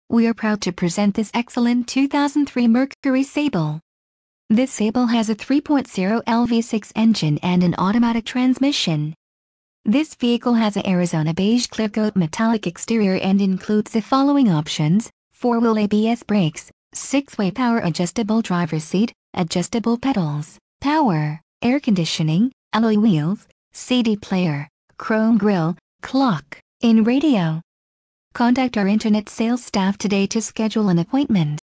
2003 Mercury Sable Park Chrysler Jeep
Park Chrysler Jeep

2003 Mercury Sable GS Fleet Plus
Engine: 3.0L V6
Trans: Automatic 4-Speed
Color: Arizona Beige Clearcoat Metallic
Mileage: 117,877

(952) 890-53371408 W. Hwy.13